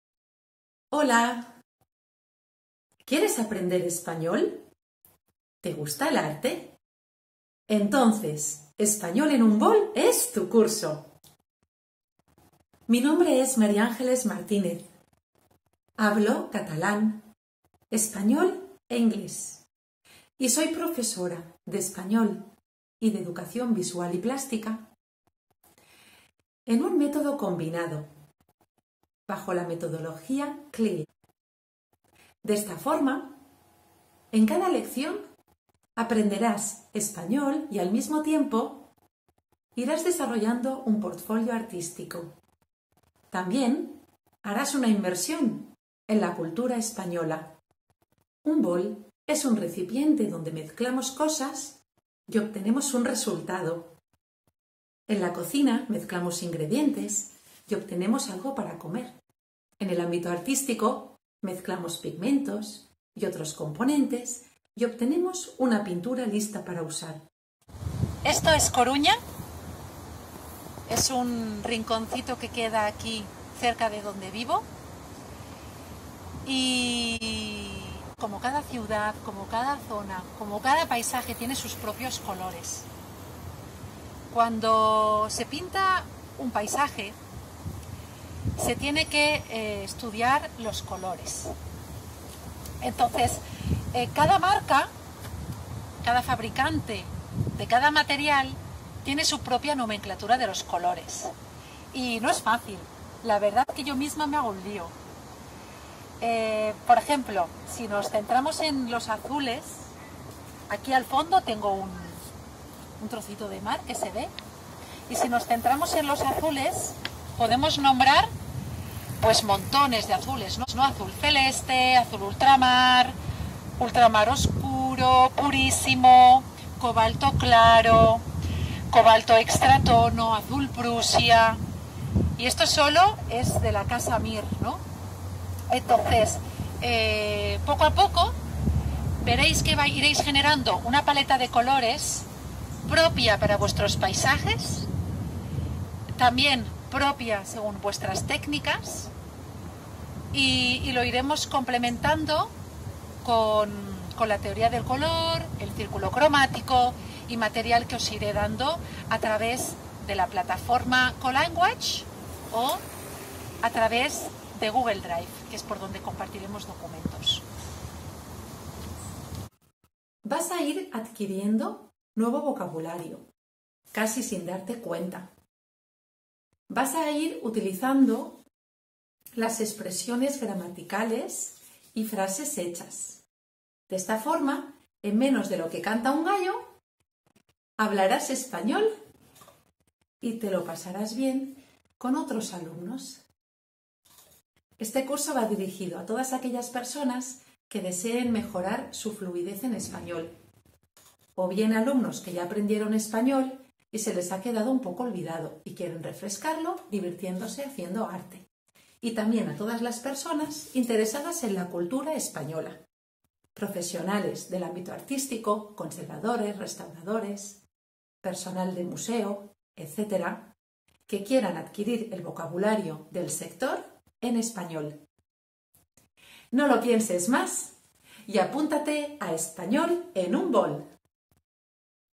Presentation video at CoLanguage by Professor María Ángeles in La Coruña, Spain
Do you want to learn Spanish doing Arts? Watch this video about the "Spanish in a bowl" lessons and start speaking Spanish with other students online in CoLanguage, as well as you have fun creating works of art. During the lessons, students will visit virtual museus and will explore Spanish culture (artists, museums, monuments...). Also, students will create an artistic Portfolio (drawings, sketches, paintings and other artcrafts). Students will learn new Spanish vocabulary, grammar and expressions. Is your Spanish a bit rusty and do you need more fluency? Do you enjoy painting and doing activities online with other fellows (or just alone) and the teacher? Use Spanish as the communication language. It is aimed at all levels. Join the first lesson for free. Set up the frecuency of lessons as you prefer. Furthemore, you can combine these lessons with other Spanish lessons on CoLanguage to prepare for the DELE certificate.